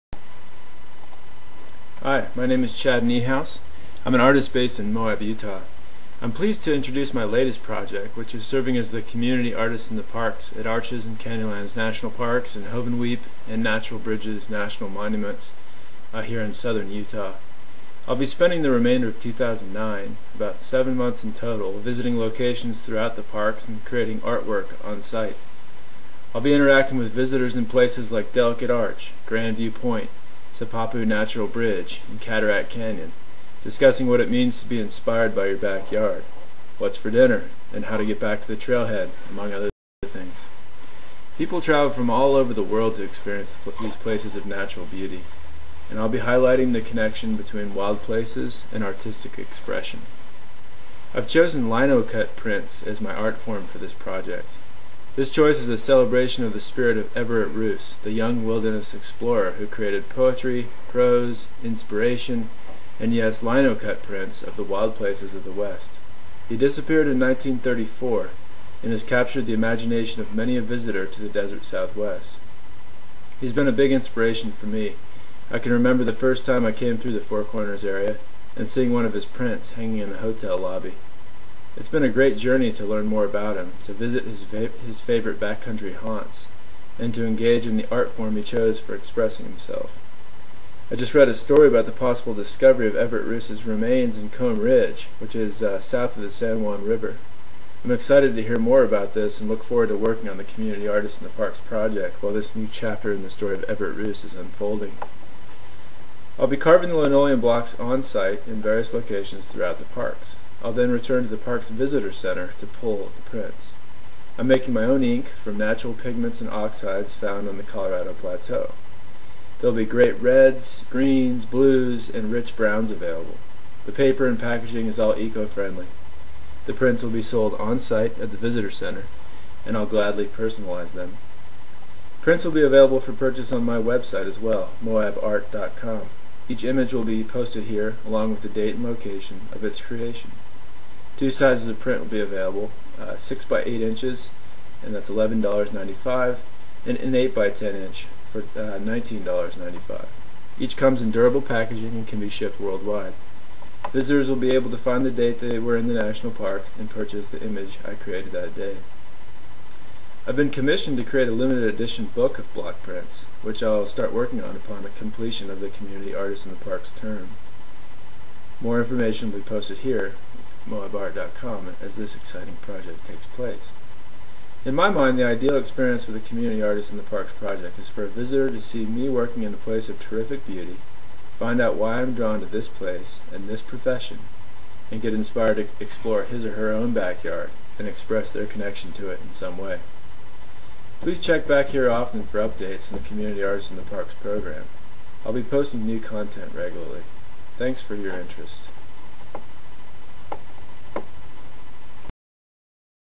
Chad Niehaus - Community Artist in the Parks - Introduction
Artist Chad Niehaus describes the Community Artist in Parks program, a 7-month art residency program at Arches, Canyonlands, Hovenweep, and Natural Bridges in southeastern Utah.